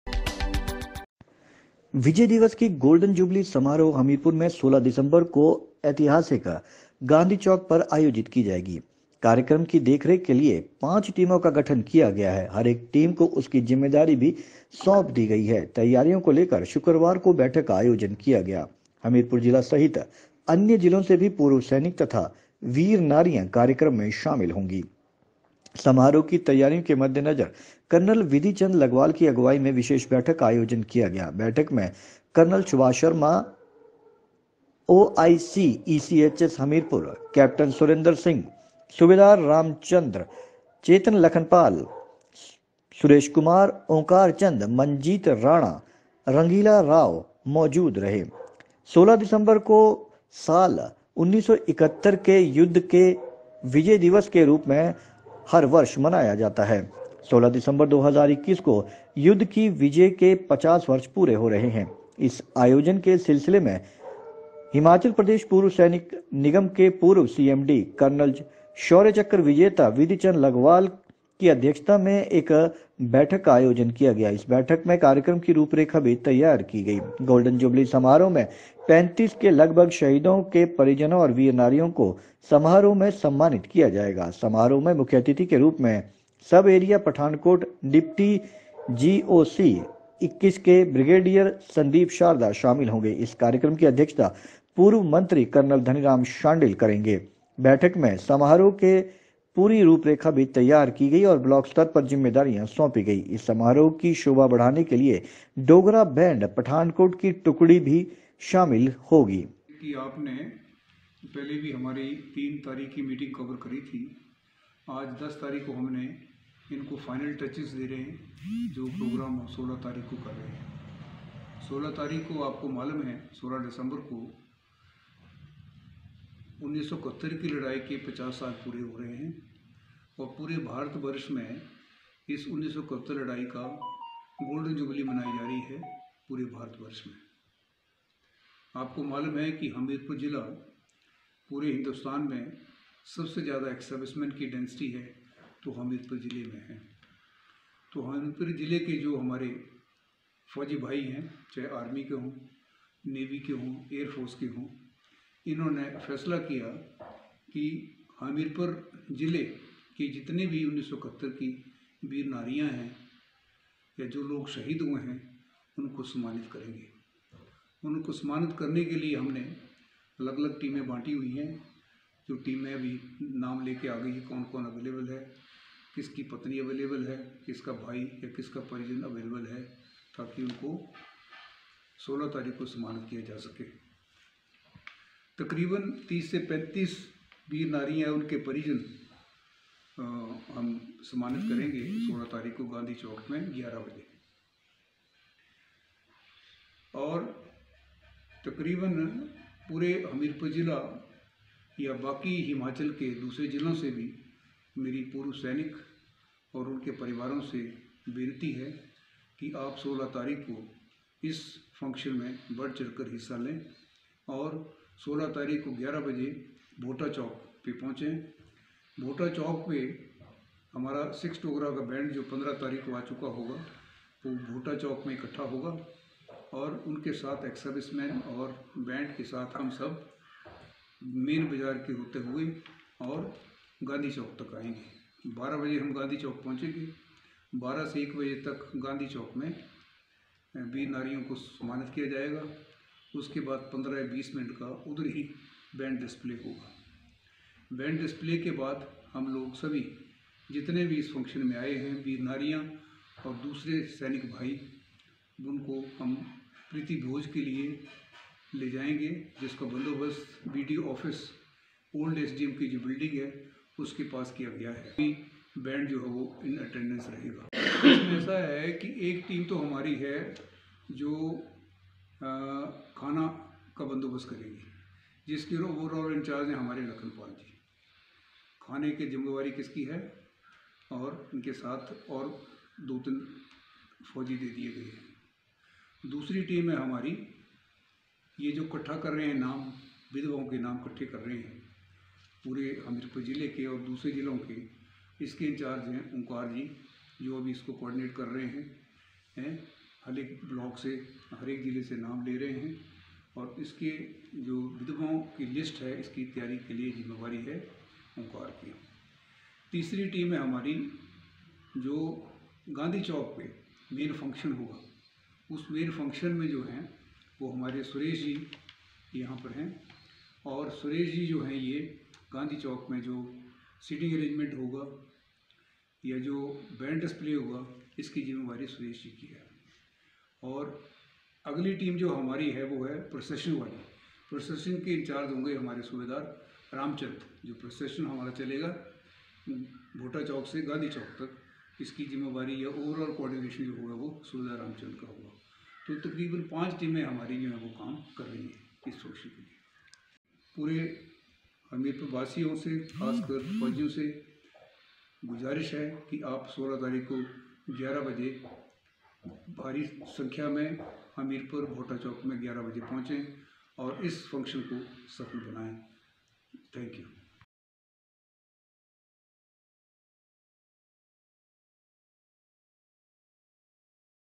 hamirpur Ex-servicemen and Veer women will be honored at Gandhi Chowk
पांच टीमें देखेंगी विजय दिवस गोल्डन जुबली समारोह की  तैयारियां
गांधी चौक पर सम्मानित होंगे पूर्व सैनिक व वीर नारियां
16 दिसंबर को भोटा चौक से निकलेगी गांधी चौक पर पहुंचेगा काफिला

हमीरपुर।
विजय दिवस की गोल्डन जुबली समारोह हमीरपुर में 16 दिसंबर को हमीरपुर के ऐतिहासिक गांधी चौक पर मनाया जाएगा। कार्यक्रम की देखरेख के लिए पांच टीमों का गठन किया गया है। हरेक टीम को उसकी जिम्मेवारी सौंप दी गई है।